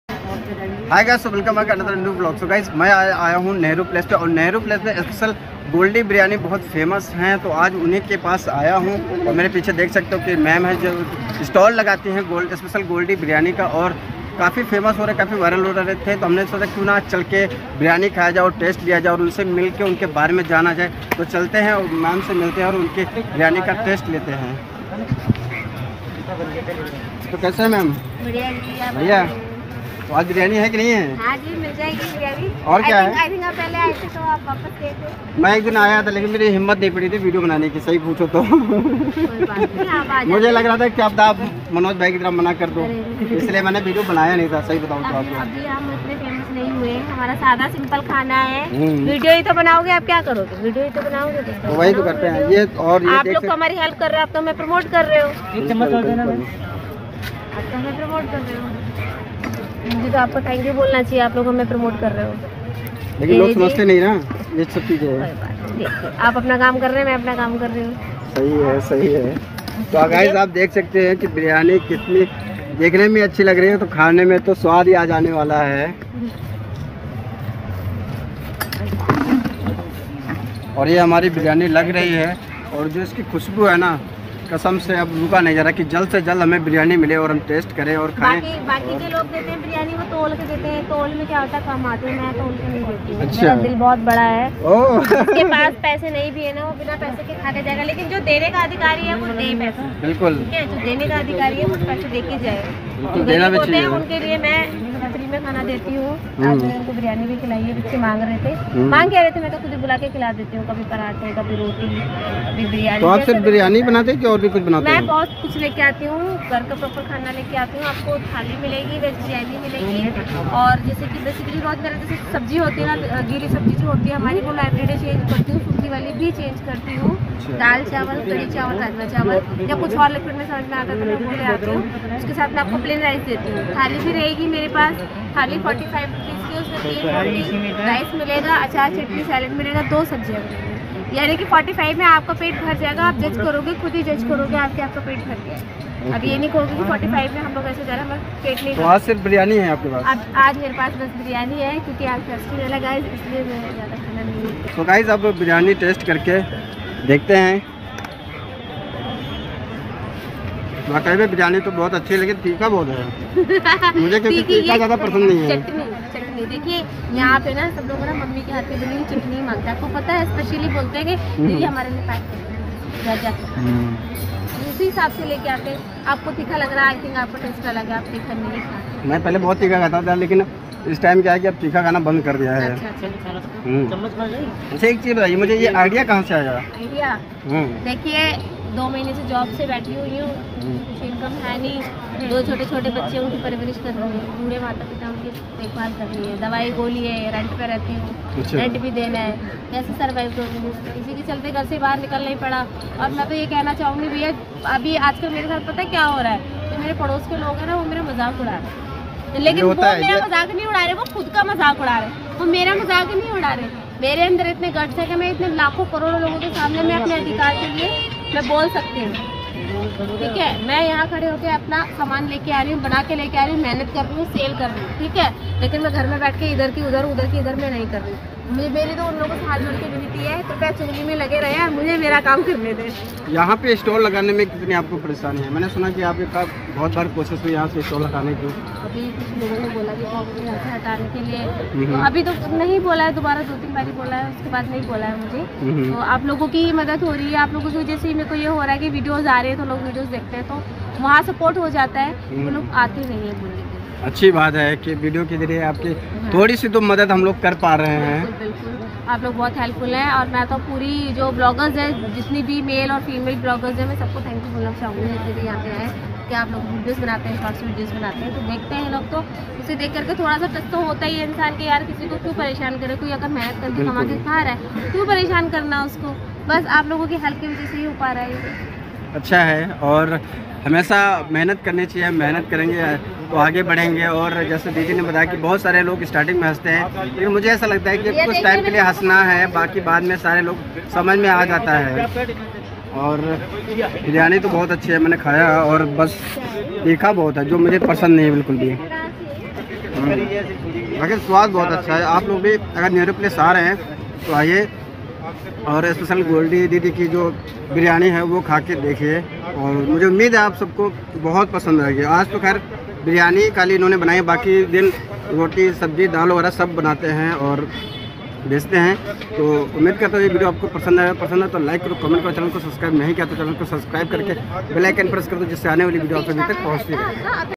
Special Goldy Biryani Nehru Place | Two sister Selling special Goldy biryani in Nehru place
Guys Thankyou So Much For watching this video

         Thankyou for Watching This Vlogs